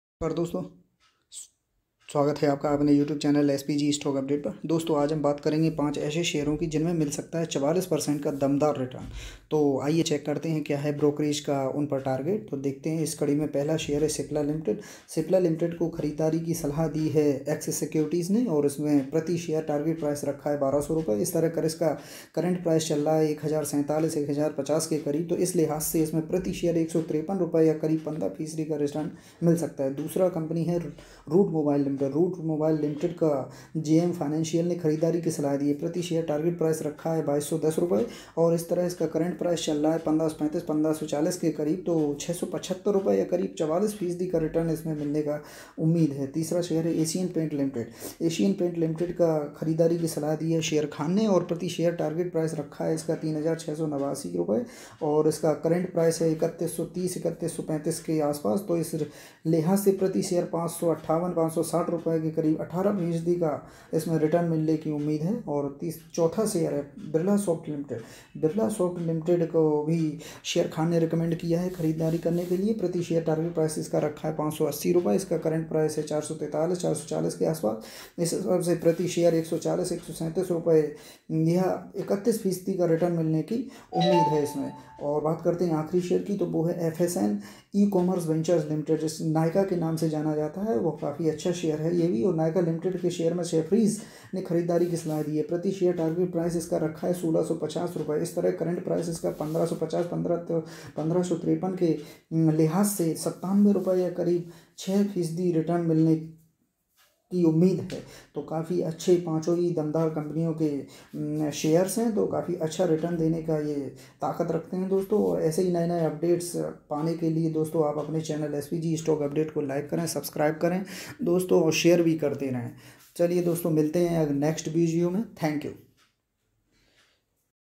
Stocks to buy: इन शेयरों में मिल सकता है 44% तक दमदार रिटर्न, देखो ब्रोकरेज का टारगेट
Stocks to buy: इन शेयरों में मिल सकता है 44% तक दमदार रिटर्न, चेक करें ब्रोकरेज का टारगेट